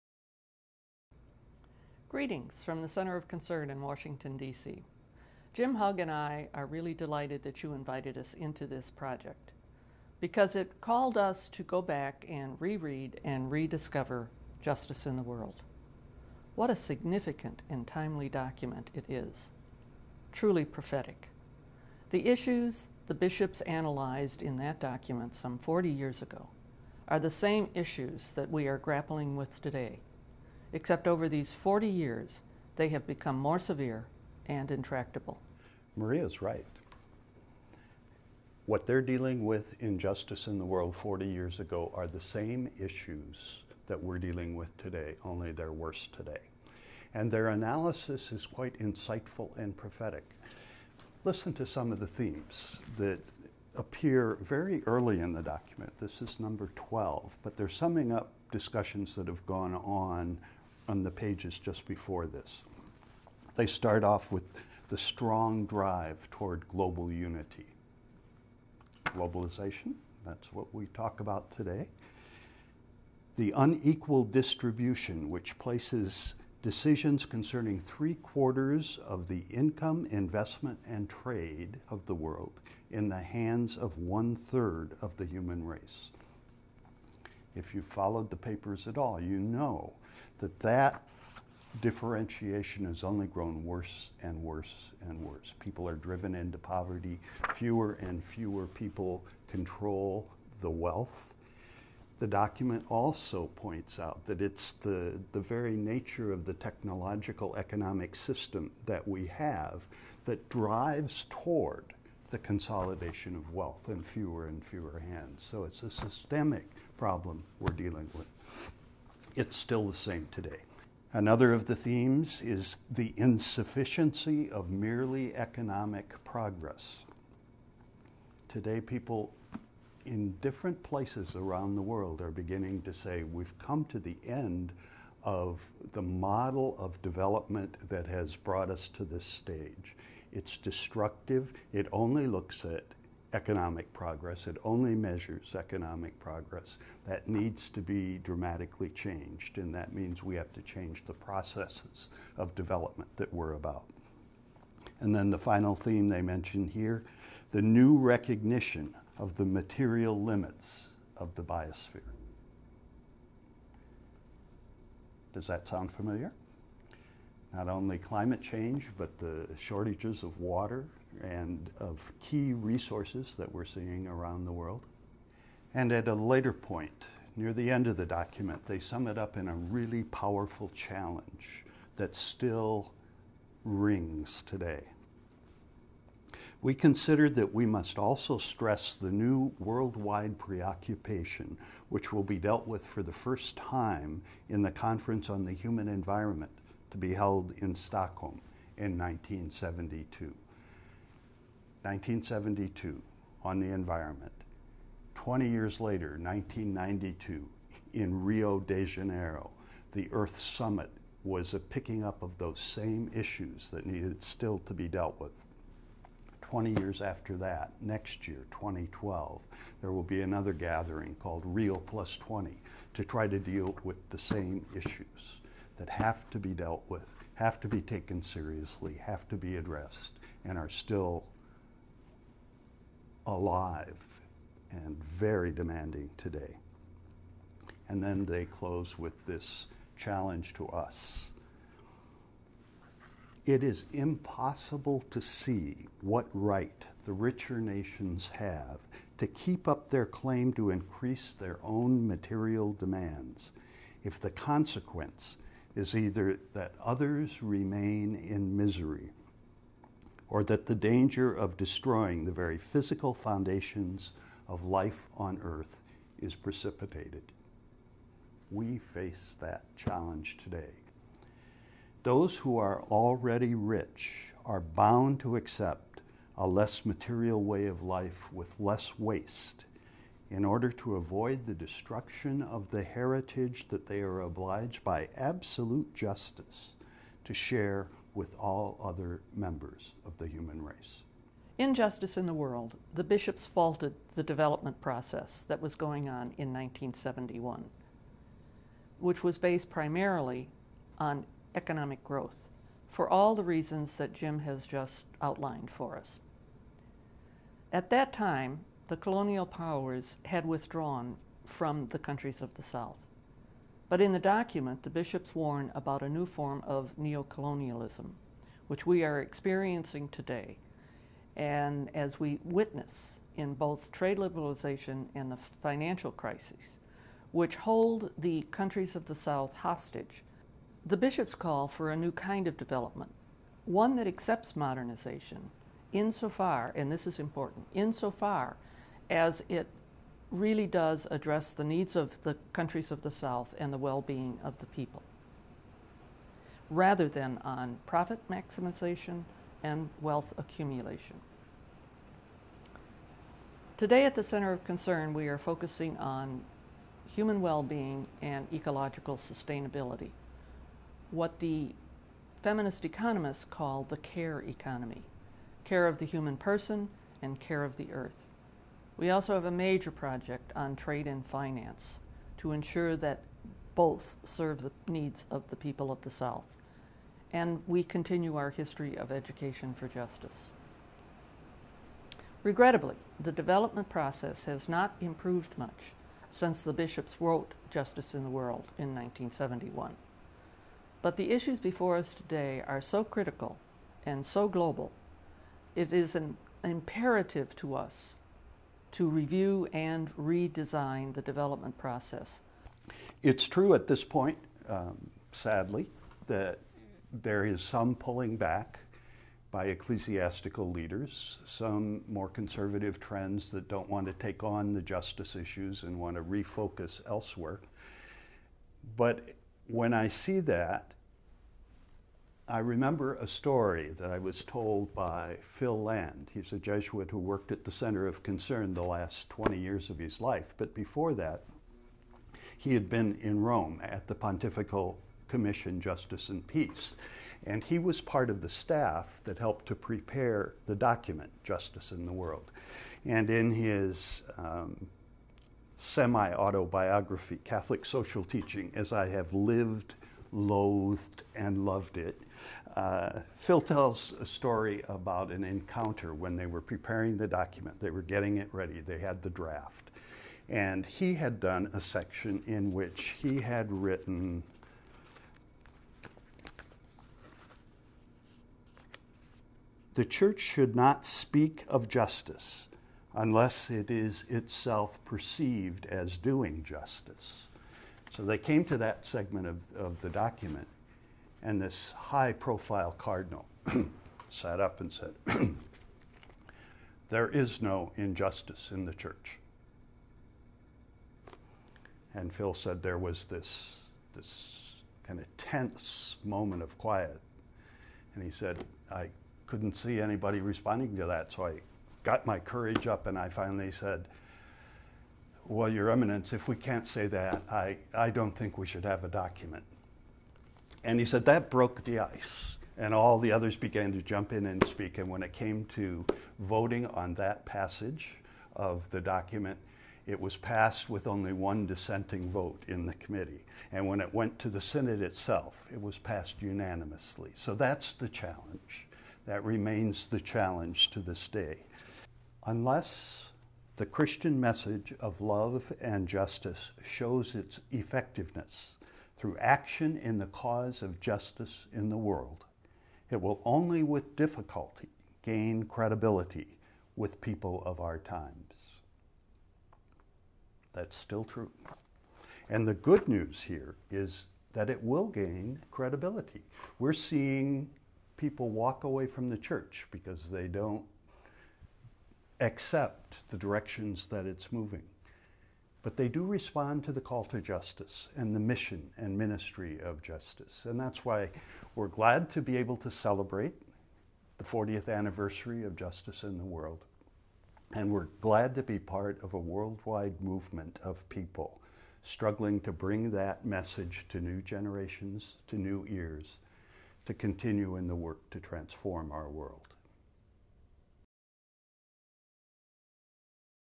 Justice in the World - 40 years later
The Center of Concern's Maria Riley, O.P., and Jim Hug, S.J. reflect on the 1971 Bishops' Synod document, Justice in the World.  It is disturbingly accurate to the world situation today.